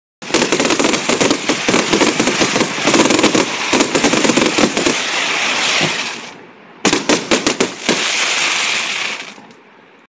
Fireworks Demo Show (Cake) 728
100 shots 1.2 inch Blue mine with crackling chry.